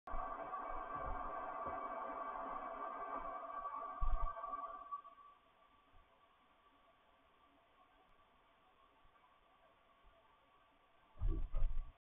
Haunted Doll...Demonic voice says “Lutka” means Doll in Croatian
We started having some strange things happen after I bought this creepy doll. I decided to put her in a closet with a camera on her.  I received a notification of sound through the Wyze camera app.  I did not catch any movement, but the camera did pick up what sounds like some kind of interference at the beginning of the video clip and at the very end of the clip is a very deep demonic sounding voice. Two syllables, have no clue what it says!